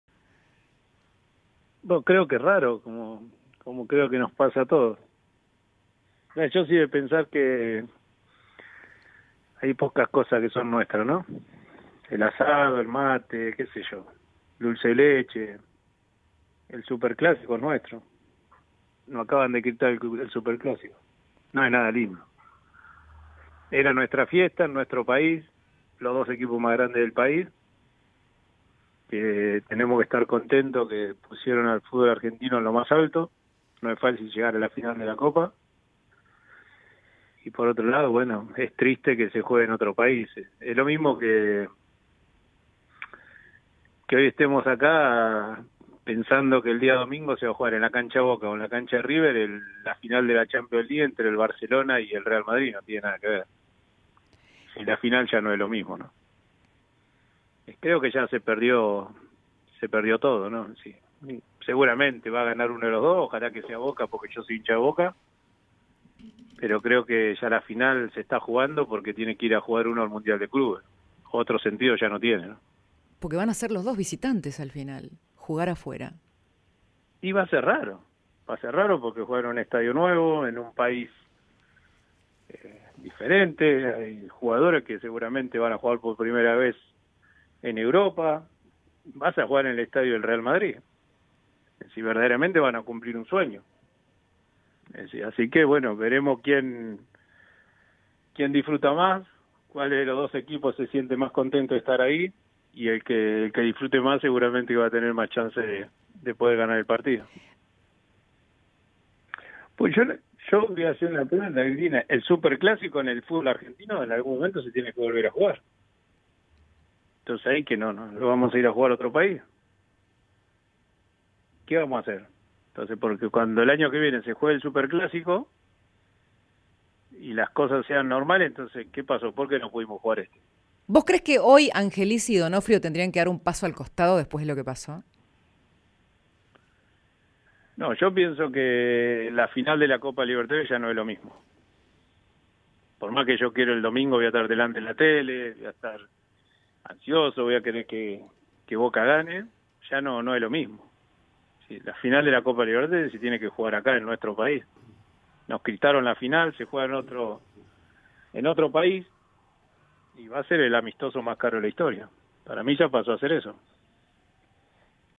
Riquelme clama contra la Superfinal en el Bernabéu
"Nos quitaron la final, va a ser el amistoso más caro de la historia". Así de contundente respondió el mítico Juan Román Riquelme.